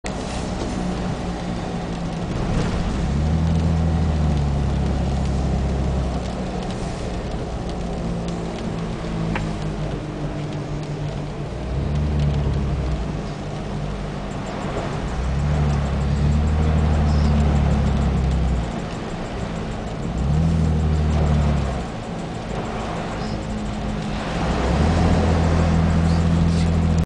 07 Silverado & FJ
Down by San Ysidro airport enjoying the rain and celebrating Christians new born Valeria